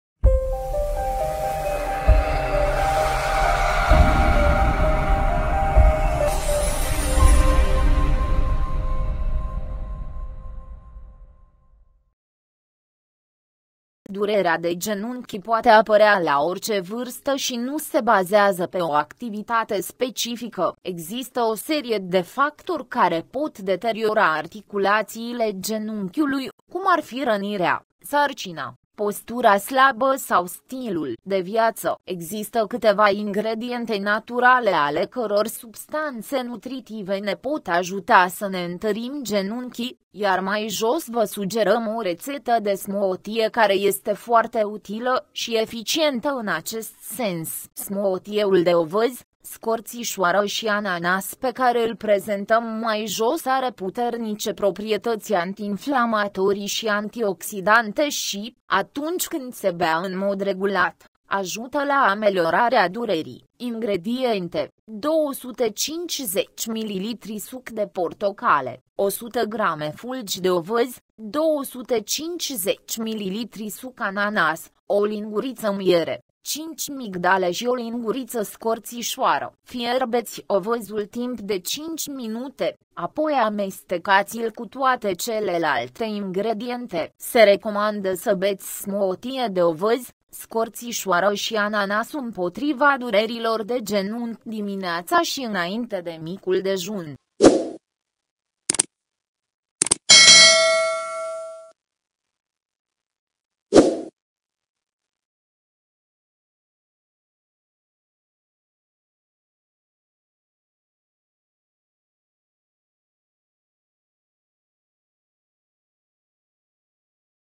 Rețeta pentru smoothie ul de ovăz, scorțișoară și ananas care regenerează cartilajul la genunchi
Rețeta pentru smoothie-ul de ovăz, scorțișoară și ananas care regenerează cartilajul și ameliorează durerile de genunchi
Durerea de genunchi poate apărea la orice vârstă și nu se bazează pe o activitate specifică.
Există o serie de factori care pot deteriora articulațiile genunchiului, cum ar fi rănirea, sarcina, postura slabă sau stilul
 de viață.

Există câteva ingrediente naturale ale căror substanțe nutritive ne pot ajuta să ne întărim genunchii , iar mai jos vă
sugerăm o rețetă de smoothie care este foarte utilă și eficientă în acest sens.

Smoothieul de ovăz, scorțișoară și ananas pe care îl prezentăm mai jos are puternice proprietăți antiinflamatorii și
antioxidante și, atunci când se bea în mod regulat, ajută la ameliorarea durerii.

Ingrediente: 250 mililitri suc de portocale, 100 grame fulgi de ovăz, 250 mililitri suc ananas, o linguriță miere, 5 migdale și o
linguriță scorțișoară.

Fierbeți ovăzul timp de 5 minute, apoi amestecați-l cu toate celelalte ingrediente.
Se recomandă să beți smoothie de ovăz, scorțișoară și ananas împotriva durerilor de genunchi dimineața și înainte de micul
dejun.